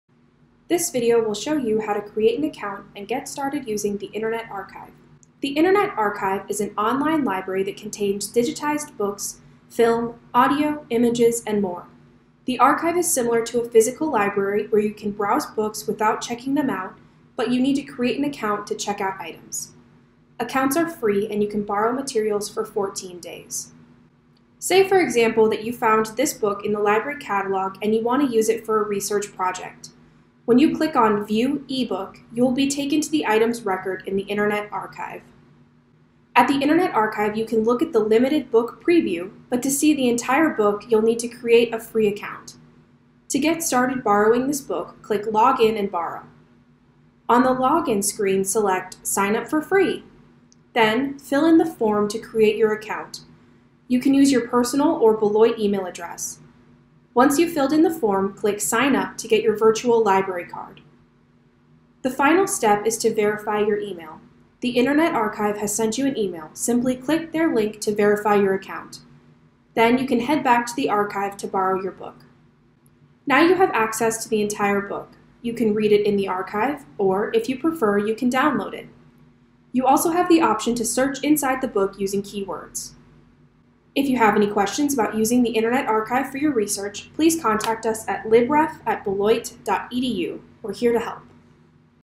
Creating an Internet Archive Account
Brief tutorial on creating an account to access content from the Internet Archive.